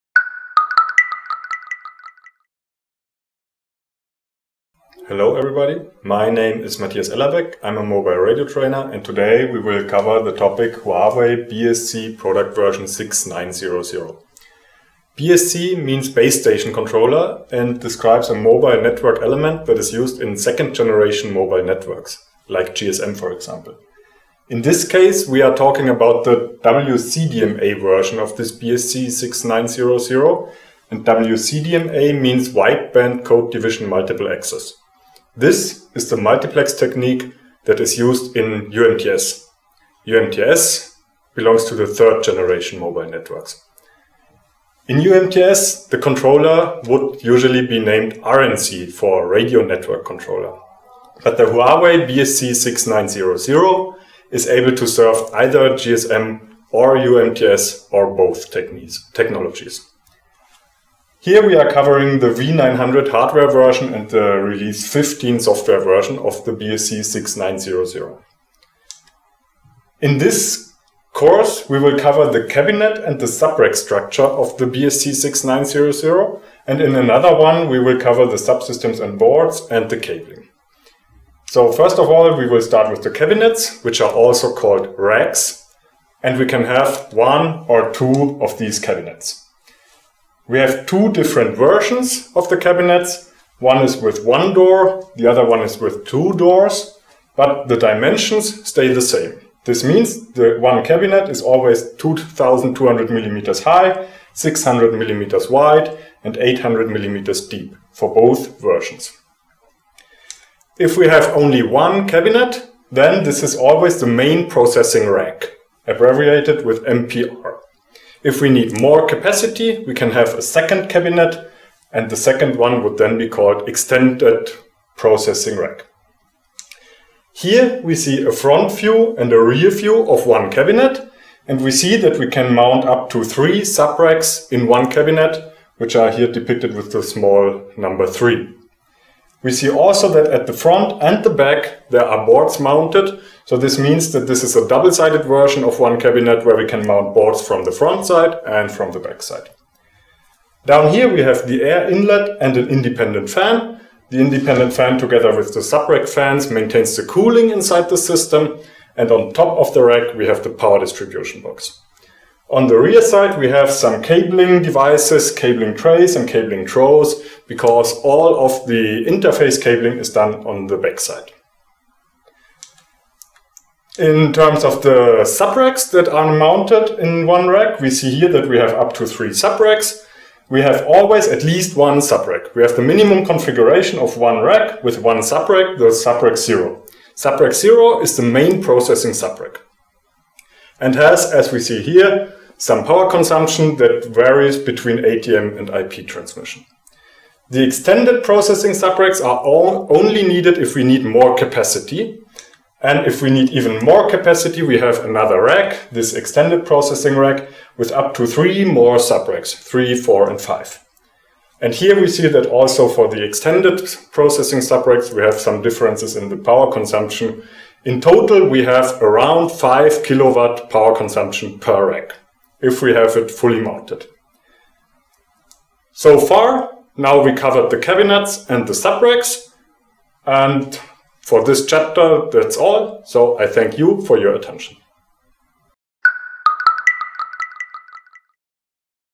WCDMA Huawei BSC6900 Courseware Sample
This Video is just an sample of elearning what Huawei is providing today. With this there are several other courses that Huawei is providing . For more details contact Bikas-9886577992